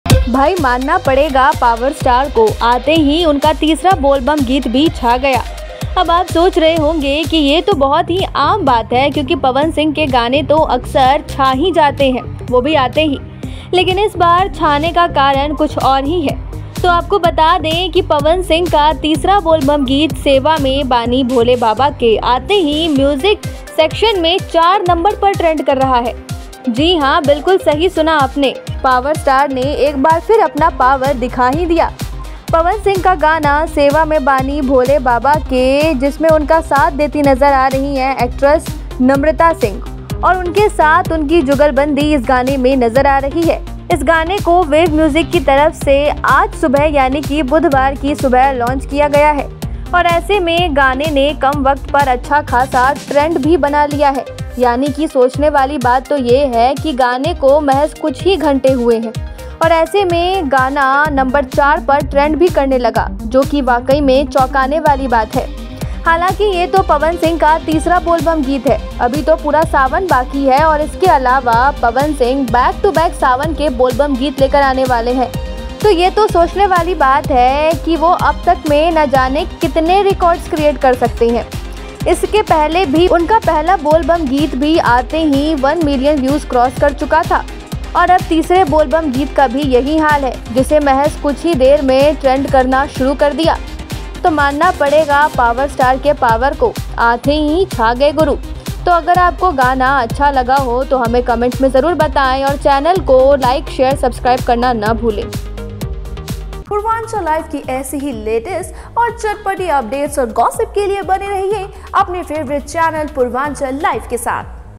एक बार फिर पावर स्टार मर गए बाज़ी आते ही ट्रेंड करने लगा तीसरा बोलबम गीत....PL
पवन सिंह के तीसरे बोल बम गीत ने रिलीज होते ही मार ली बाजी, आते ही छा गया यह बोल बम गीत...